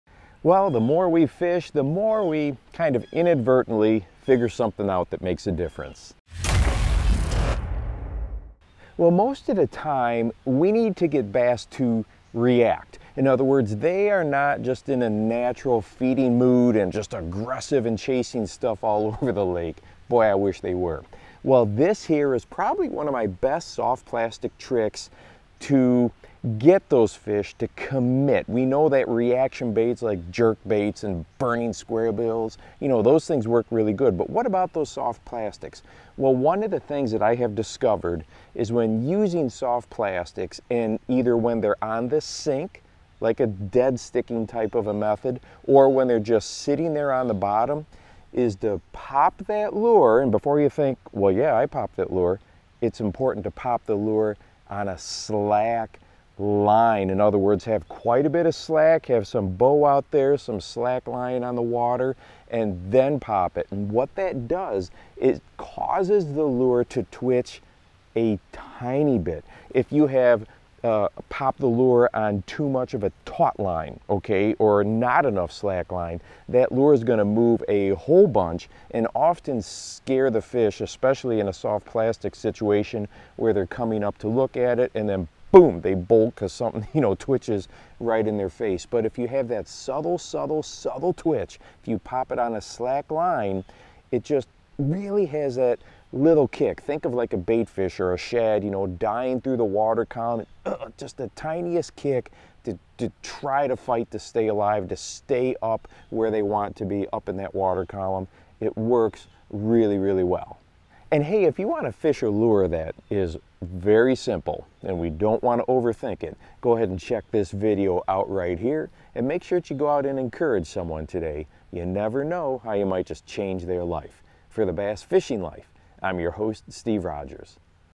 My #1 Soft Plastic Trick for Bass Fishing
#9 of 30 Videos in 30 Days Challenge: Time on the water is the greatest teacher. This simple and very effective addition to your soft plastic bass fishing can be a game-changer for you. Links to some of my favorite products are below.

Like Coffee? This is the coffee company my wife and I own, specializing in the bass angler.

Chapters
0:00 How We Learn
0:10 What We Have to Do Most of the Time
0:38 What I Discovered
0:52 The Key to Making This Work
1:02 What it Does

Favorite Lures of Mine